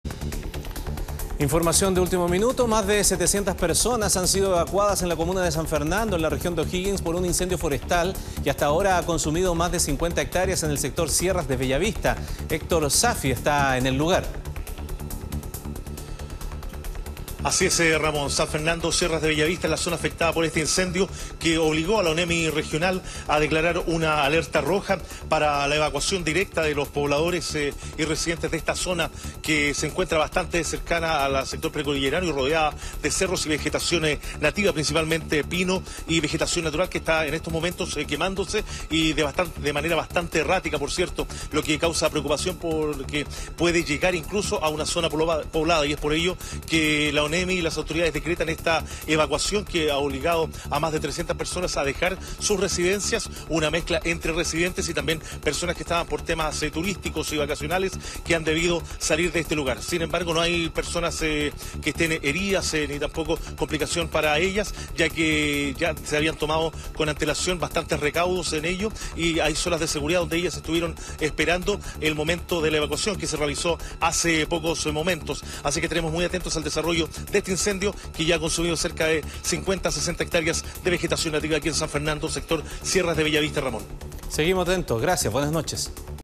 Alerta Roja por incendio forestal en San Fernando: 700 personas han sido evacuadas
A través del sistema de mensajería SAE, los habitantes del sector fueron informados de que debían abandonar sus viviendas y seguir las instrucciones de la autoridad.